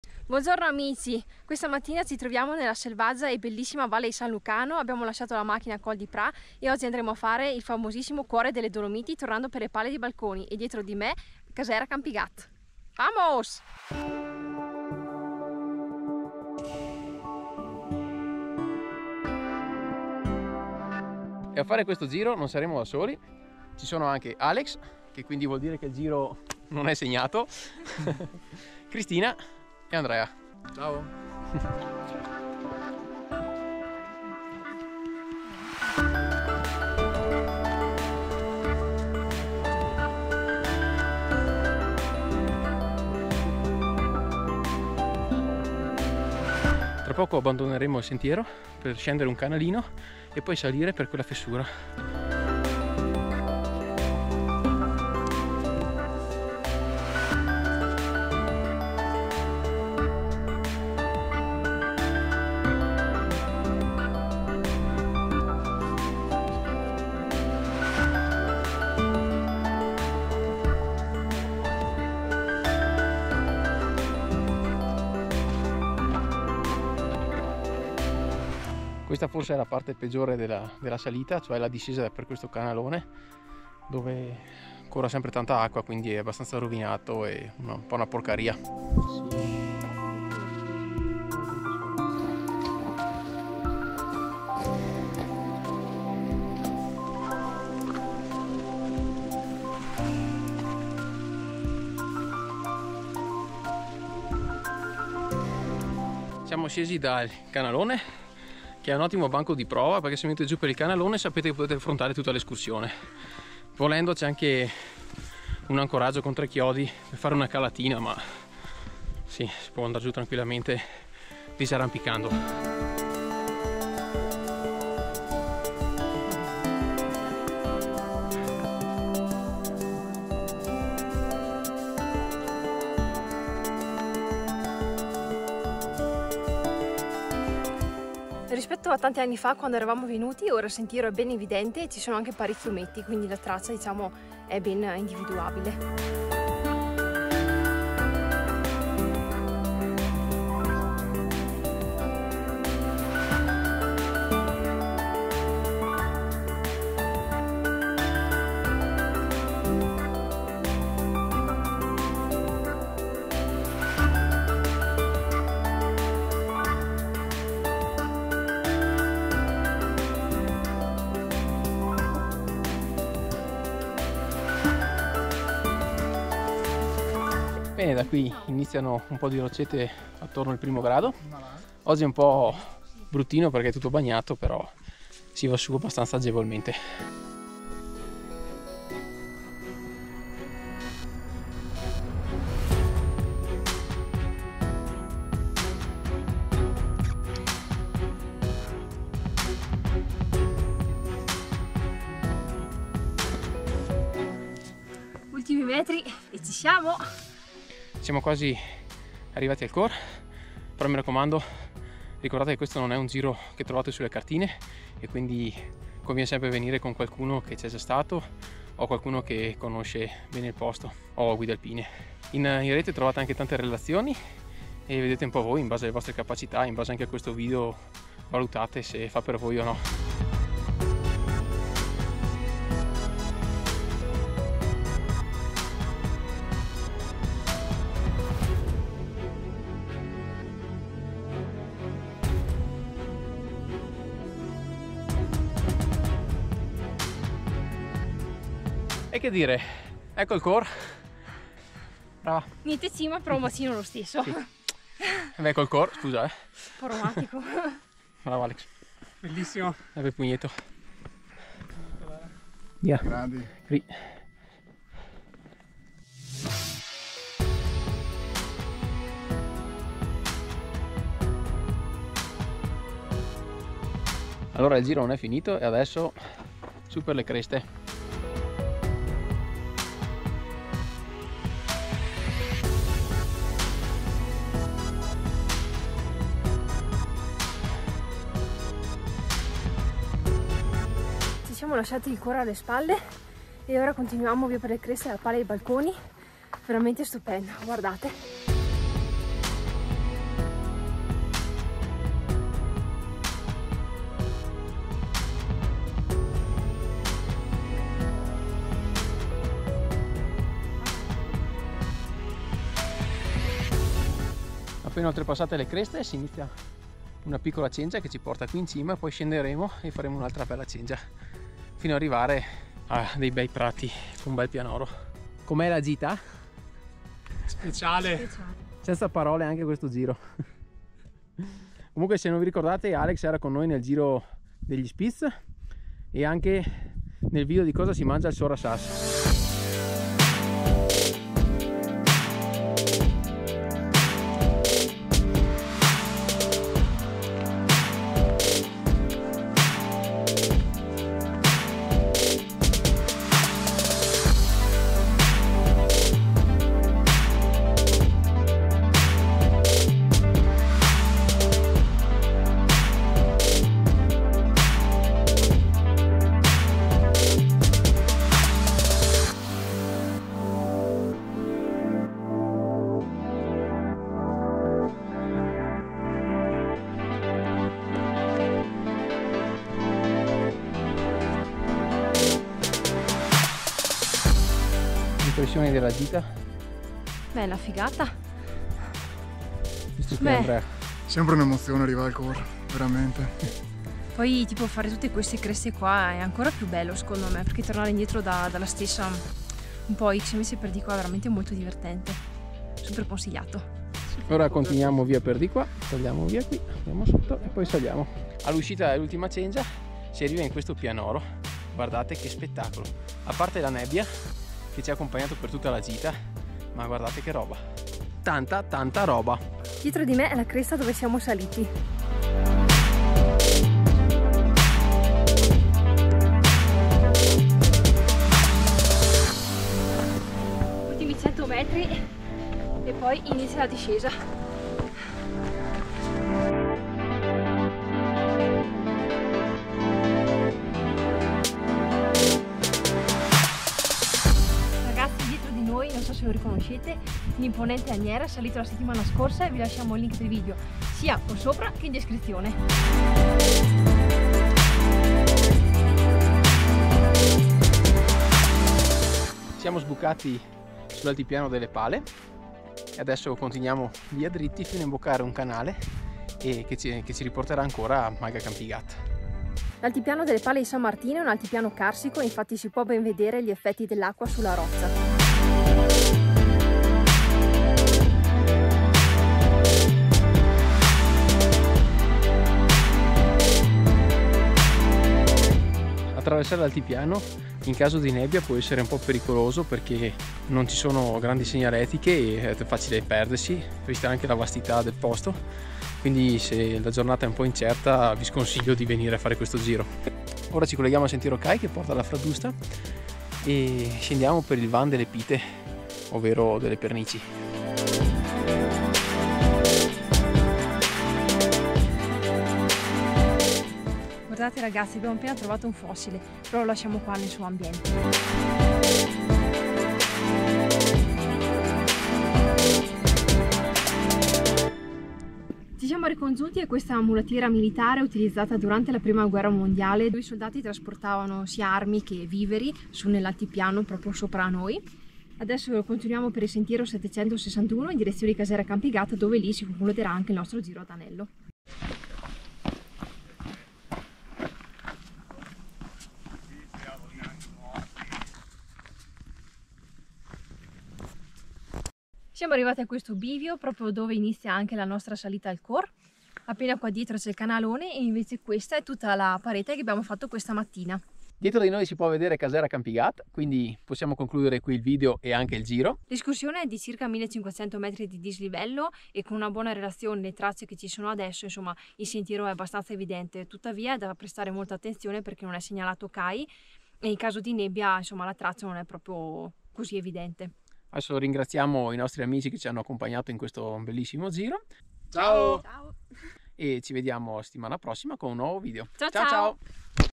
EL COR | Il cuore delle Dolomiti
El Cor è una bellissima conformazione rocciosa a forma di cuore nelle selvagge Pale di San Lucano. E' una via alpinistica tecnicamente facile ma con alcuni tratti a cui prestare attenzione, tra cui un primo canale pluviale selettivo da discendere. Da prestare attenzione alla parte finale della parete verso il Cor e la cresta esposta ed aerea sopra il cuore. Percorso meraviglioso, che sta diventando sempre più frequentato.

A/R: 7h (questo è il nostro tempo ed è solo indicativo, ognuno ha il suo passo) - 16 km
DISLIVELLO: 1500 D+
DIFFICOLTA': MEDIA

Se acquisterete tramite questo link,  per voi il prezzo non cambia e noi riceviamo una piccola percentuale. Siamo sicuri di consigliarvi solo ottimi prodotti testati da noi.